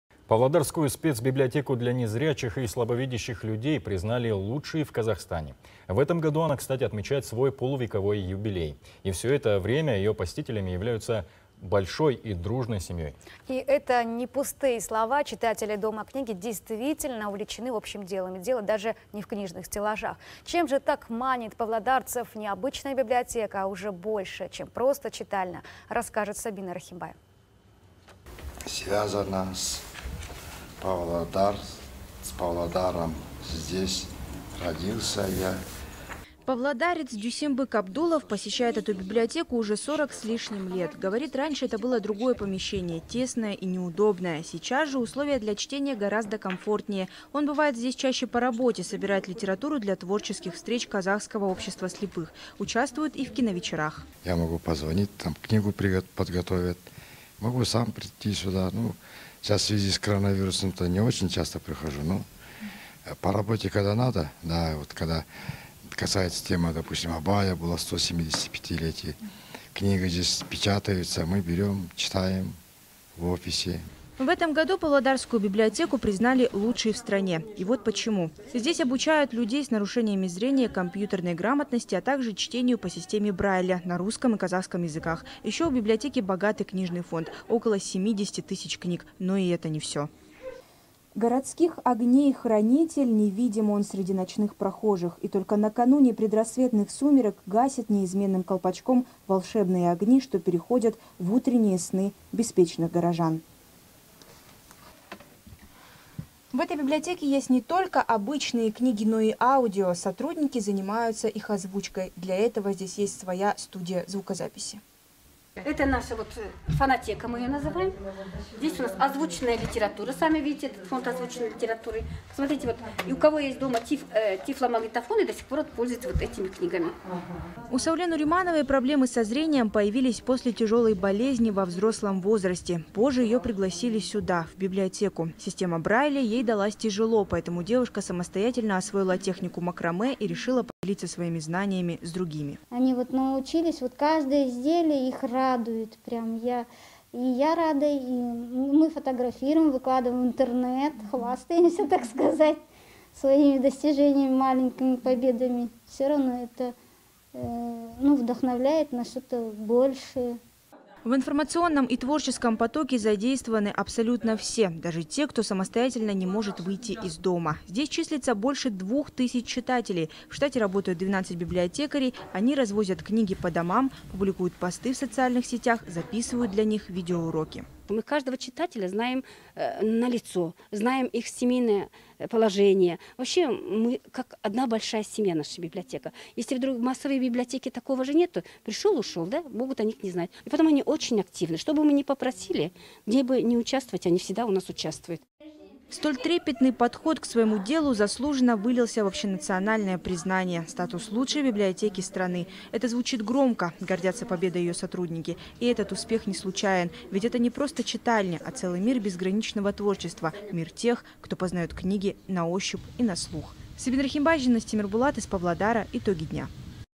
Павлодарская библиотека для незрячих и слабовидящих признана лучшей в Казахстане
Павлодарскую Спецбиблиотеку для незрячих и слабовидящих людей признали лучшей в Казахстане. В этом году она отмечает свой полувековой юбилей.

И все это время ее посетители являются одной большой и дружной семьей. Читатели Дома книги действительно увлечены общим делом, и дело даже не в книжных стеллажах.

Павлодарец Дюсембы Кабдулов посещает эту библиотеку уже 40 с лишним лет. Говорит, раньше это было другое помещение, тесное, неудобное. Сейчас же условия для чтения гораздо комфортнее. Он бывает здесь чаще по работе, собирает литературу для творческих встреч «Казахского общества слепых», участвует и в киновечерах.